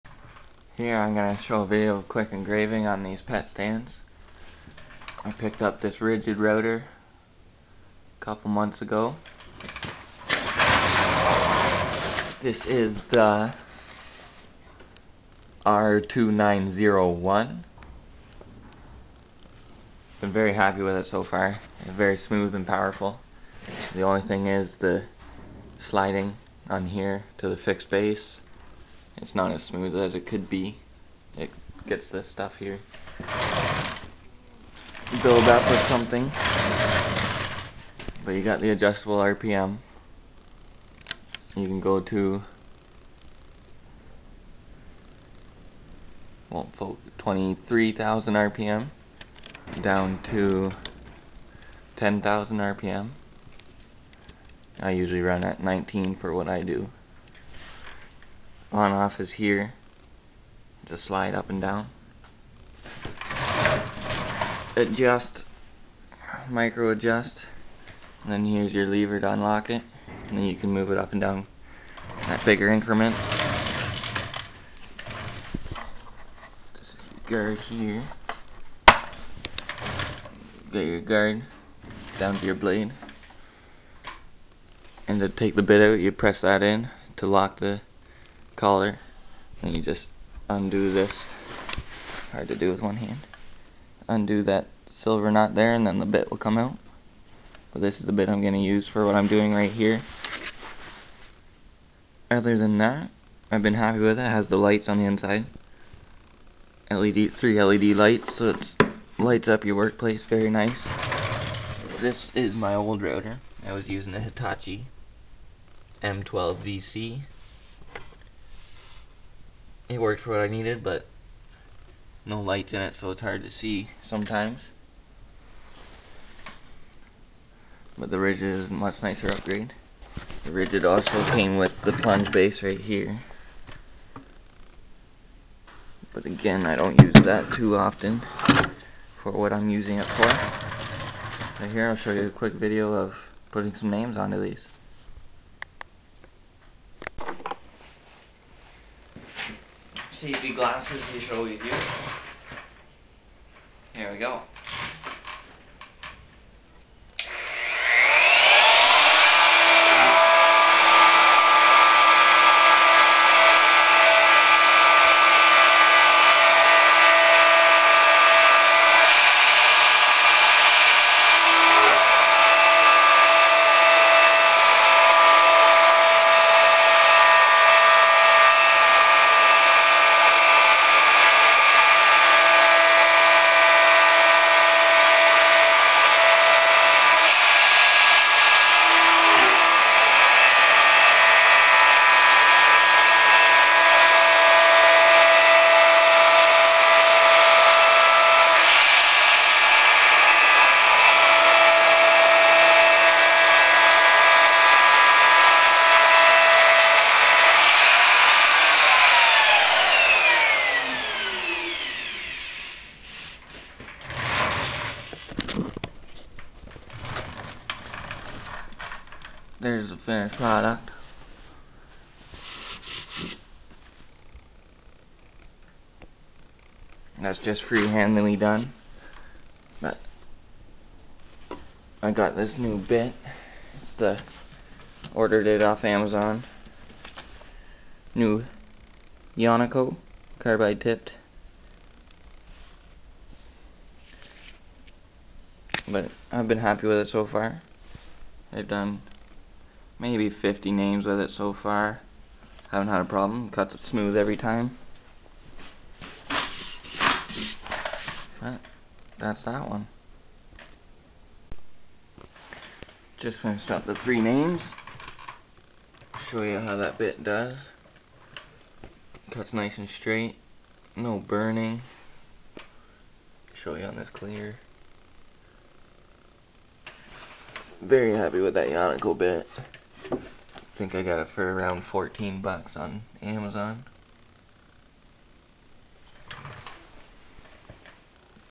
Ridgid R2901 Router with Yonico bit
Quick overview and operation of the R2901 Router, with a Yonico 90 degree bit.  Engraving some names onto pet stands.